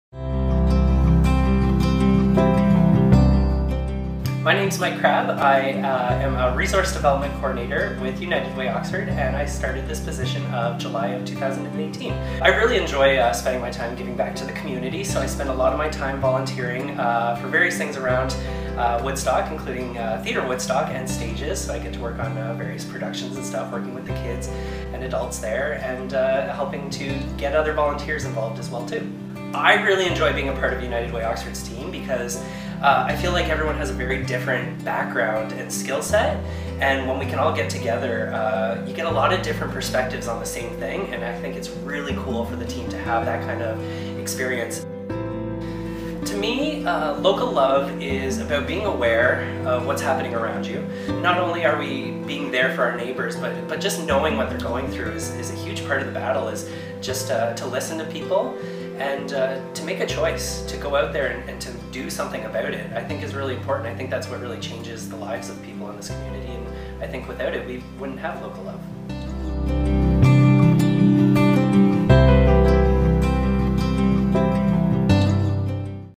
Meet the Team: Mike
Meet the team at United Way Oxford and learn what is important us.